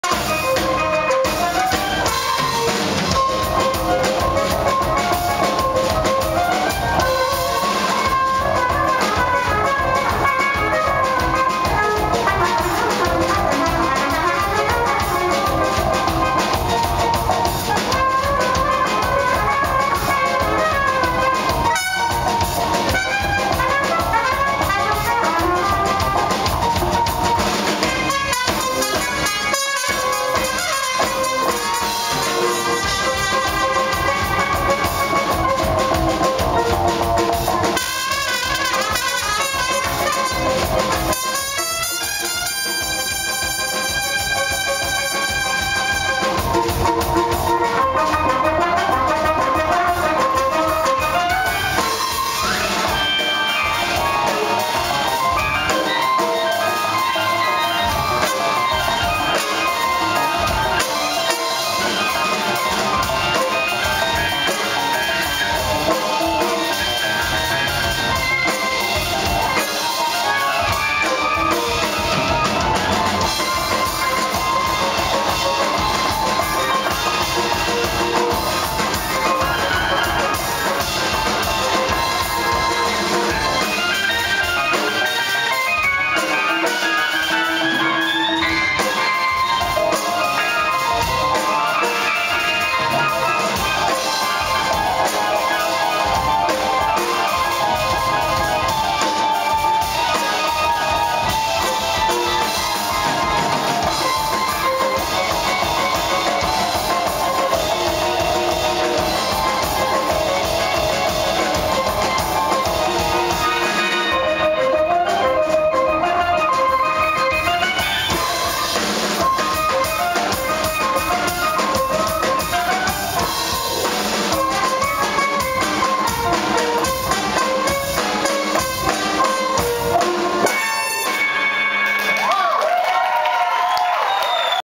Opa Cupa at the W.O.W. Hall
Opa Cupa at the W.O.W. Hall, Eugene, Oregon, 6 novembre 2008.
Cesare Dell'Anna - trumpet; Sergio Quagliarella - batteria; Mauro Tre - keyboard;
with Fabrice Martinez (violin) and Djordje Stijepovic (bass) from Fishtank Ensemble. Will add song title if I learn it.